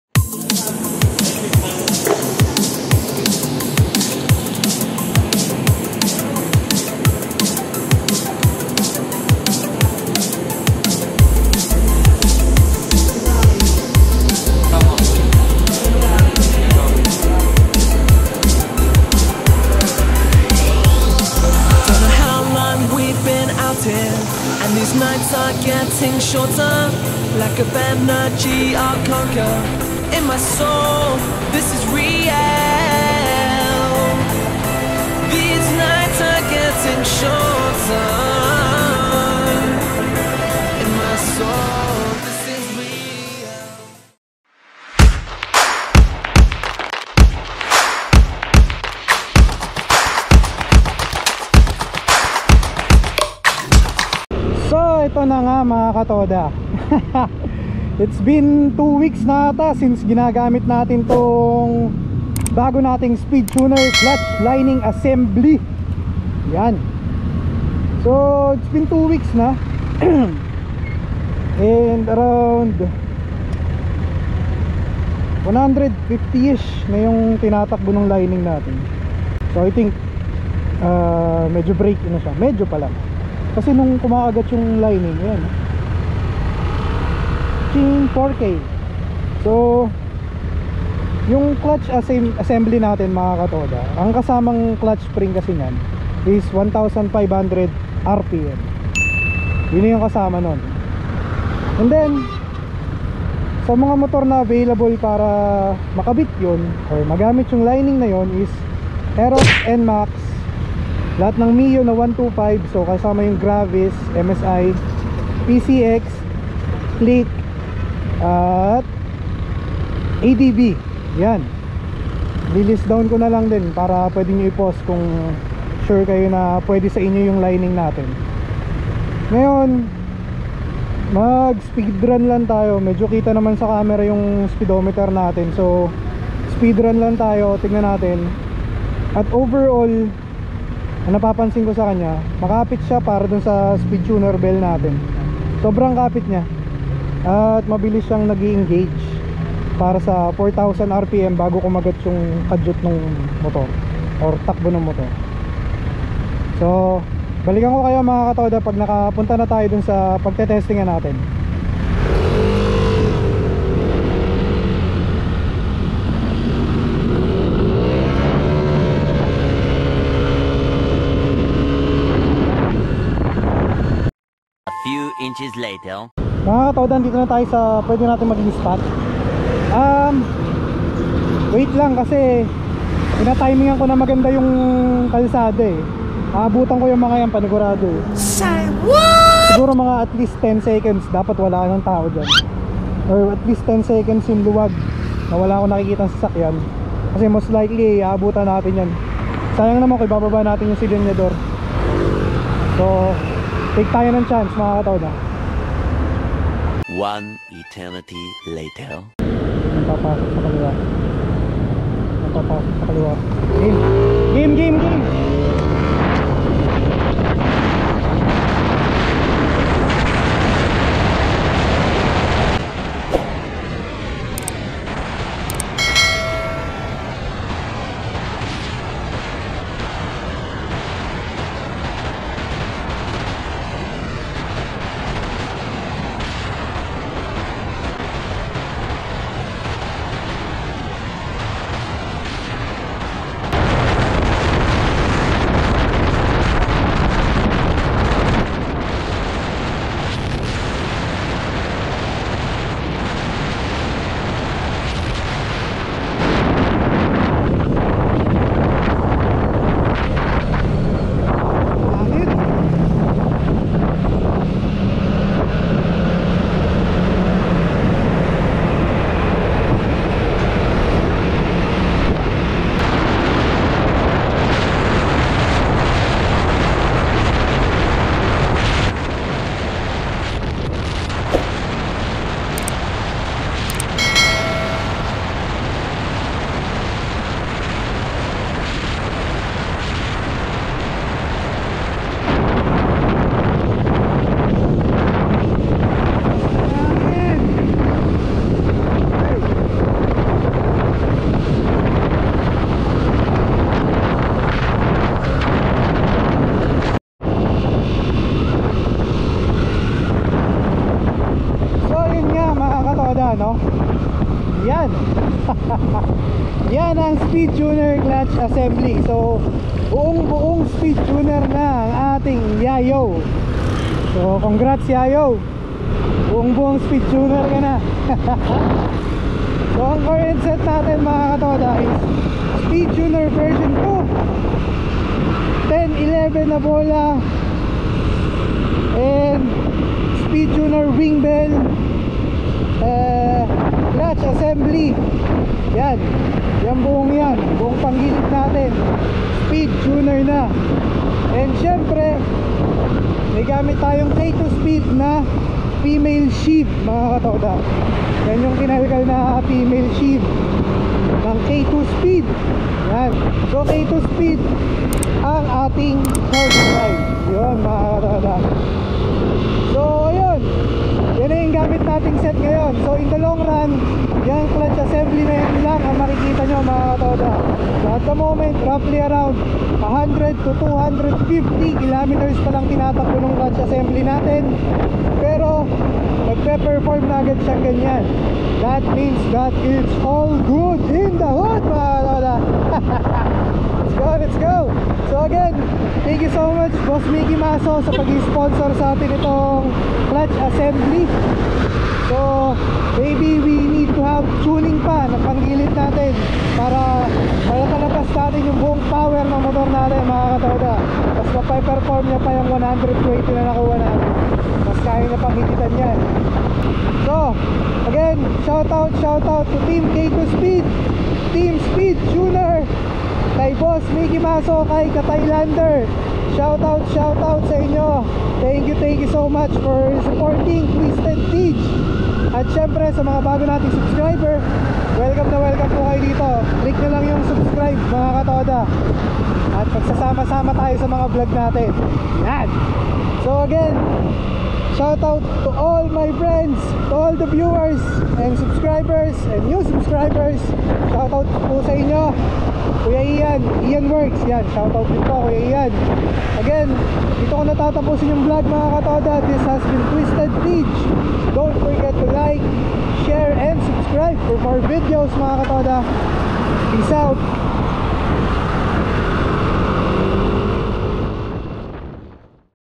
Speed Tuner clutch assembly | Mickey Mazo MM888
CVT Set: Speed Tuner by Mickey Mazo
CVT Set configuration
Pulley & DF: Speed Tuner v2
Bell: Speed Tuner
Fly Ball: 10 / 11
Center Spring: 1500rpm
Clutch Spring: 1500rpm
Clutch Lining: Speed Tuner Clutch Assembly
Torque Drive: K2 Speed Touring profile female sheave

Engine Setup
K2Speed Piston Dome 58mm(STOCK SIZE)
K2Speed Stage 1 Camshaft
JVT Valve Spring
Injector: 150cc 10holes
Throttle Body: Stock